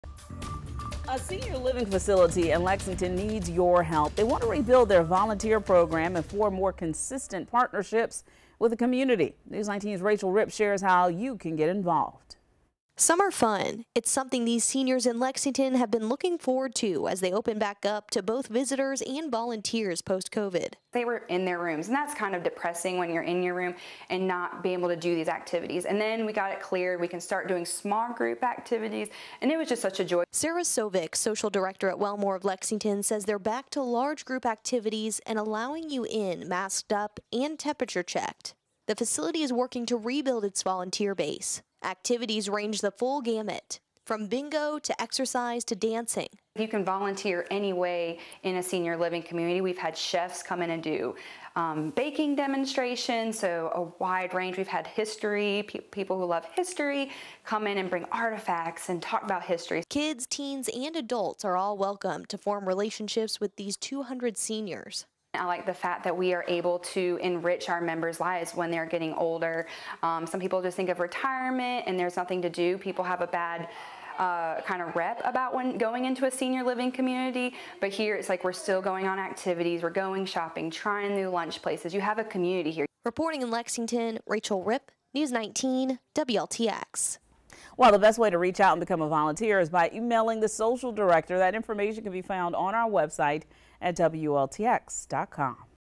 A senior living facility in Lexington is seeking volunteers
The facility is working to rebuild its volunteer base.
Activities range from the full gamut. From bingo, to exercise, to dancing.